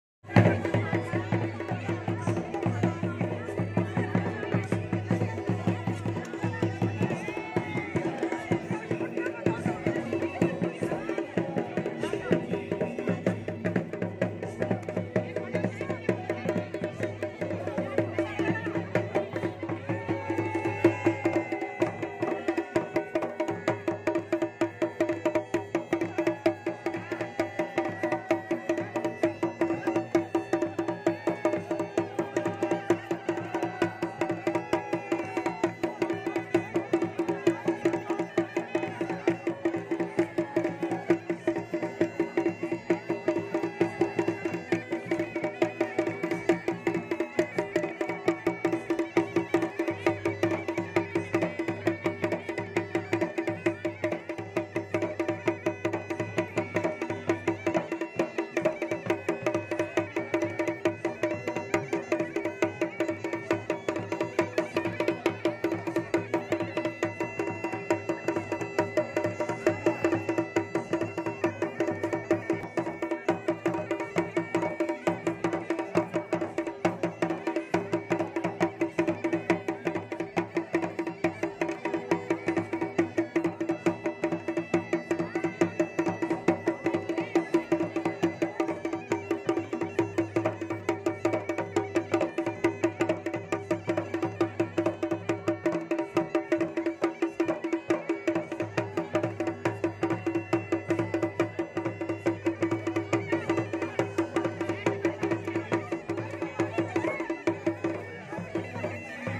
Cultural Program में ढोल दमो के साथ डांस का आनंद लेते हुए 🙏🌹♥️🌹 | Uttarakhand Dance
कल्चरल प्रोग्राम में ढोल दमो के साथ डांस का आनंद लेते हुए